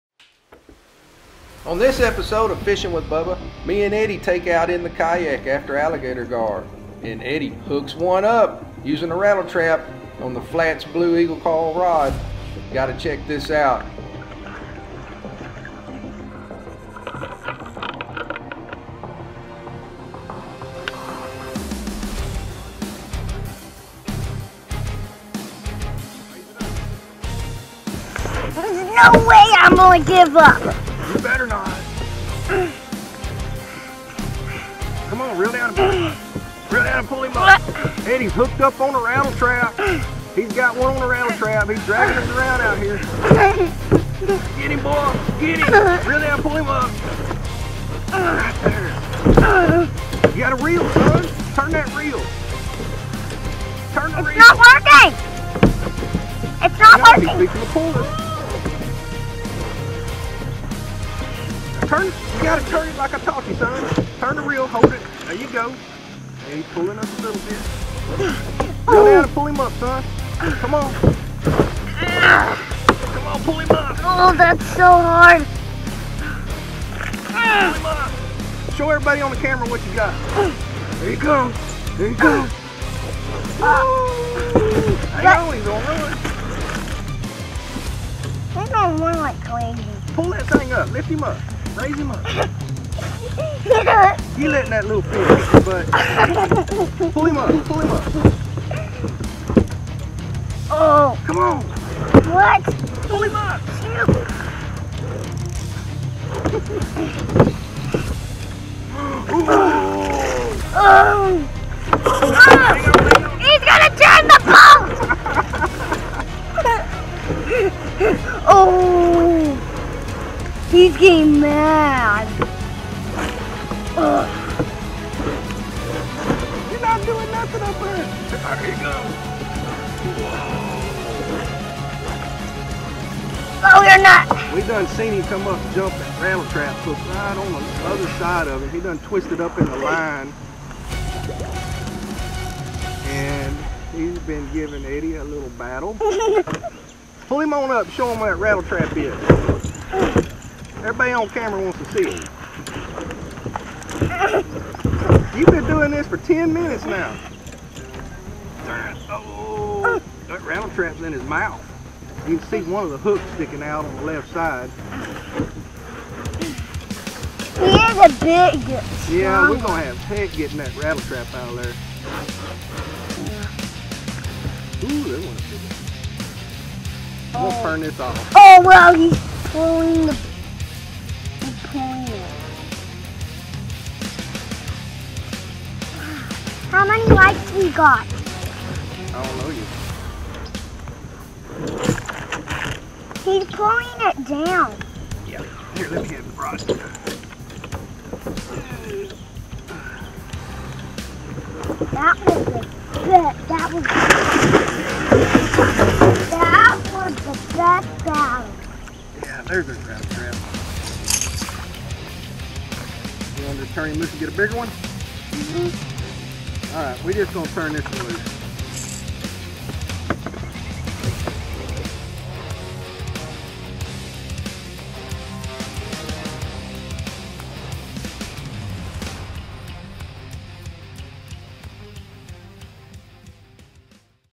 Eddie Fishing a Big Alligator Gar From The Kayak
Eddie and his dad Capt Bubba Bedre are hooked up on a big alligator gar while fishing a 1oz Rat-L-Trap from there Jackson Kayak.
Equipment used
Penn torque 7500
Spider Wire 100lb braid
Bill Lewis 1oz rattletrap
EagleClaw Flats blue Inshore rod
Trokar 4/0 treble hooks
Jackson Kayak Big tuna
Bending Branches paddle